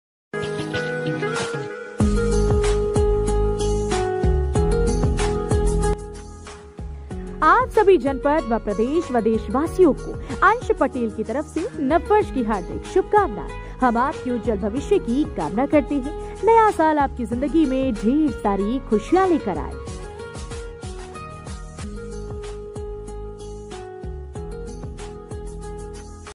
Happy New Year | Ansh Patel Etawah
आप सभी जनपद व प्रदेश व देश वासियों को

अंश पटेल

की तरफ से नव वर्ष की हार्दिक शुभकामनाएं. हम आपके उज्जवल भविष्य की कामना करते हैं. नया साल आपकी जिंदगी में ढेर सारी खुशियां लेकर आए.